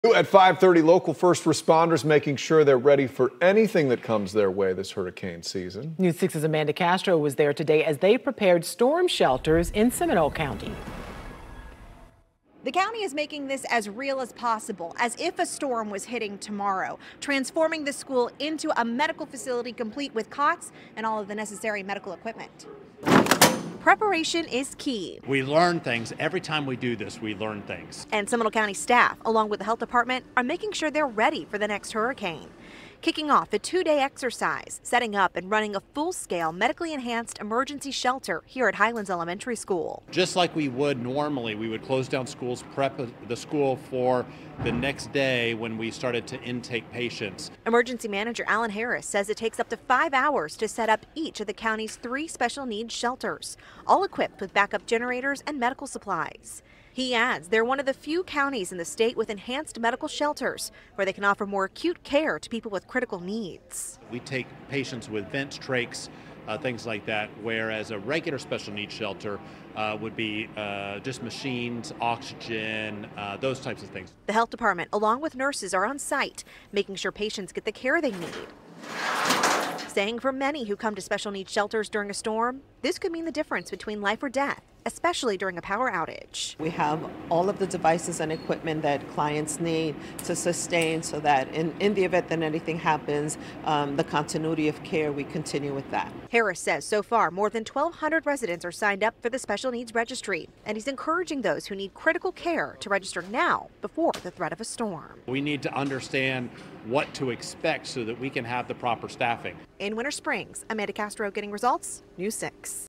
Seminole County preparing for storms with hurricane shelter training
Seminole County leaders are running a two-day, full-scale medically enhanced shelter exercise to prepare the public in the event of a severe storm or hurricane.